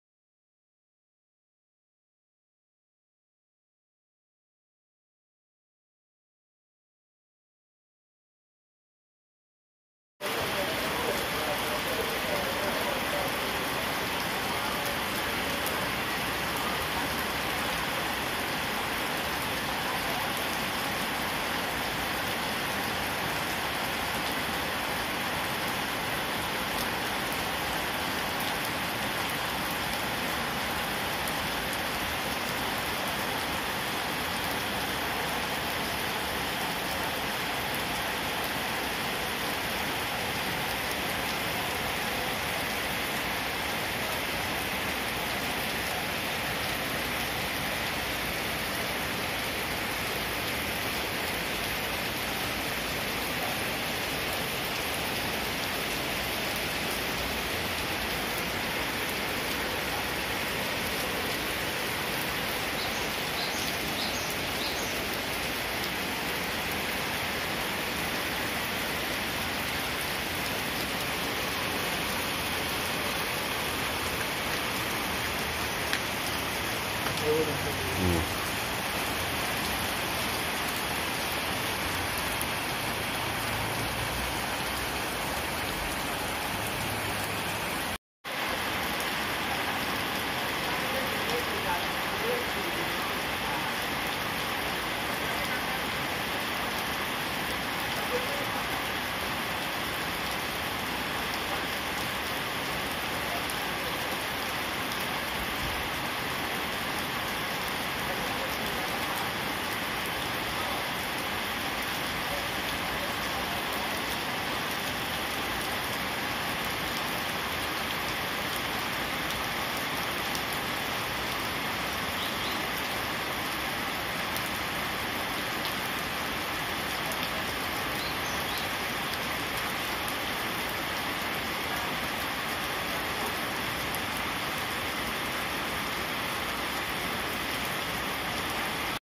Suasana Hujan Pagi Hari di kota Surabaya
Suasana Hujan pagi hari di rumah Pompa sekitar kota Surabaya